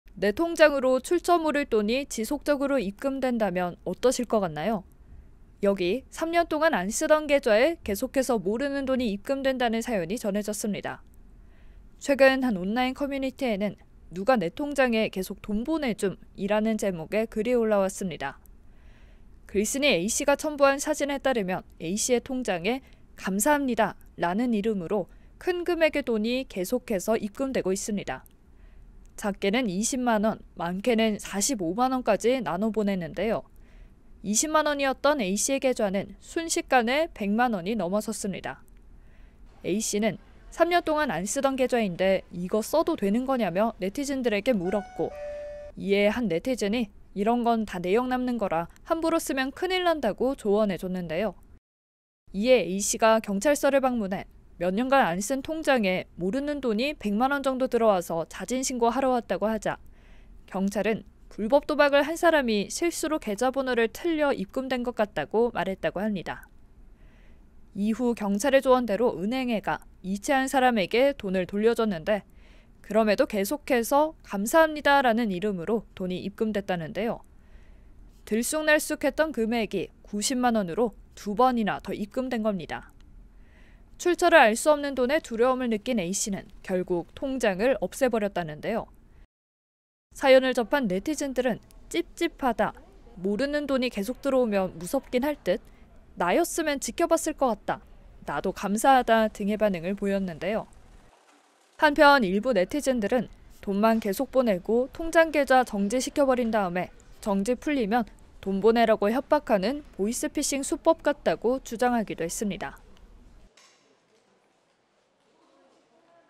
"자꾸 통장에 돈이 들어오는데..." / KNN
내 통장으로 출처 모를 돈이 지속적으로 입금된다면 어떠실 것 같나요?